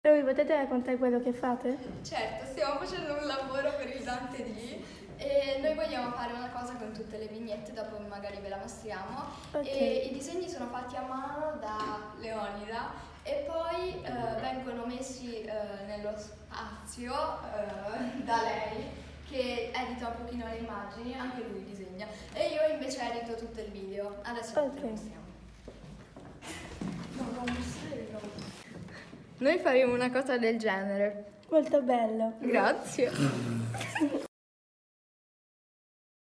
Dantedì 2023 alla II Media B della Scuola italiana Leonardo da Vinci - Parigi
La nostra inviata speciale Norma Bérranger intervista i compagni che stanno preparando i loro filmati per il concorso bandito dal Consolato di Parigi, sotto la guida del prof. Angelo Paglia